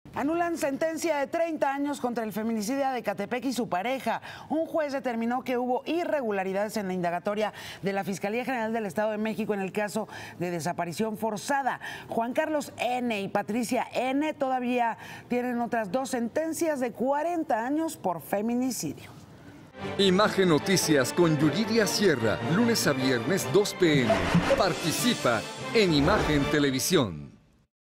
Bajan la sentencia de los 'Monstruos de Ecatepec' por irregularidades en su proceso | Yuriria Sierra
Un juez de control revocó la sentencia de 30 años de prisión contra Juan Carlos Hernández Béjar, conocido como el ‘Monstruo de Ecatepec’, y su pareja Patricia Martínez Bernal.